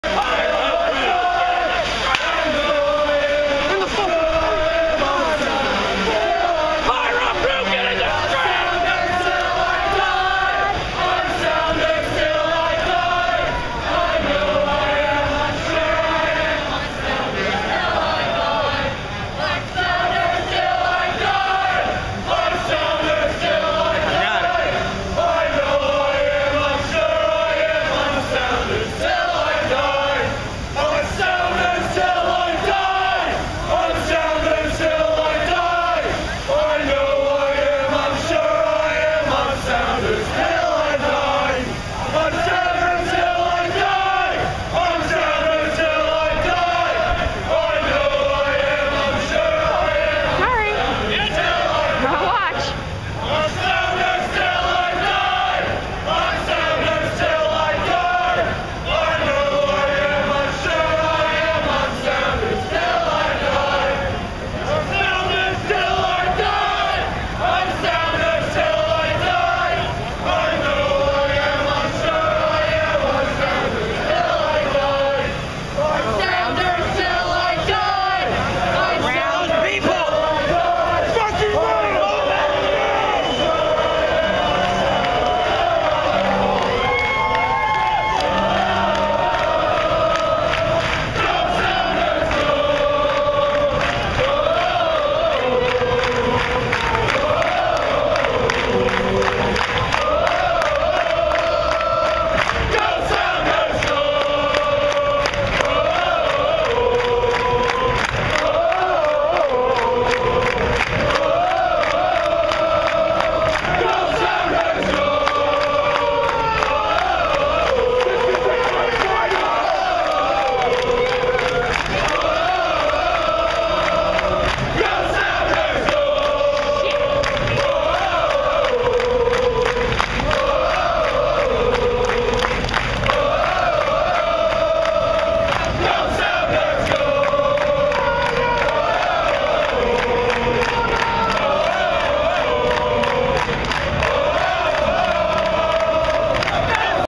ECS March to the Match USOC Final
Seattle Sounders FC 2010 USOC Champions

March to the Match for the USOC Final.  Video is a little shaky but you get the idea.